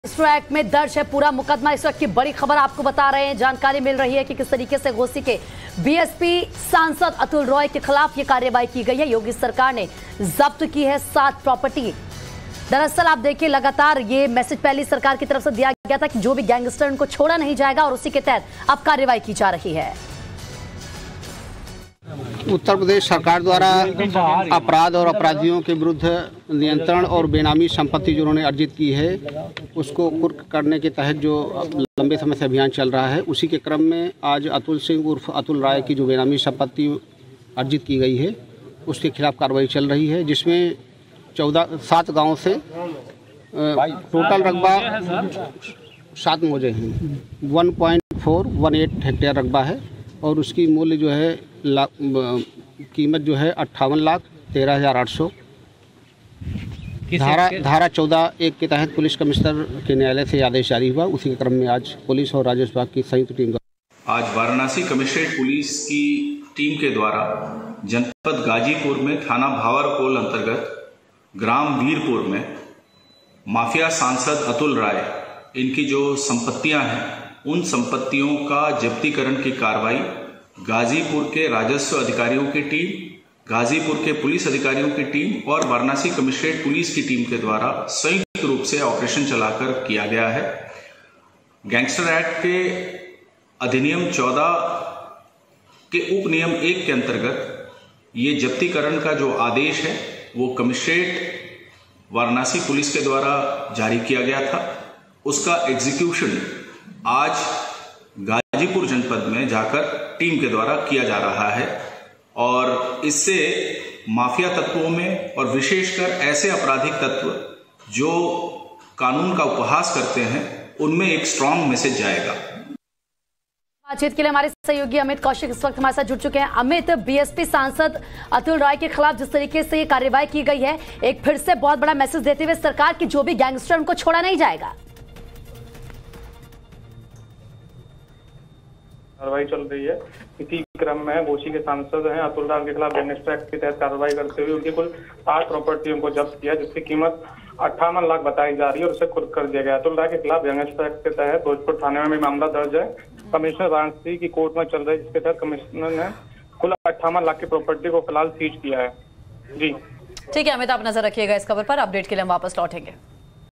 UP: BSP सांसद के खिलाफ Yogi सरकार की कार्रवाई तेज, गैंगस्टर एक्ट के तहत Atul Roy की 7 प्रोपर्टी जब्त
Uttar Pradesh Latest News : Ghosi के BSP सांसद Atul Roy के खिलाफ UP सरकार ने की कार्रवाई, गैंगस्टर एक्ट के तहत Yogi सरकार ने सांसद की 58 करोड़ की 7 बेनामी प्रोपर्टी को कुर्क कर लिया है।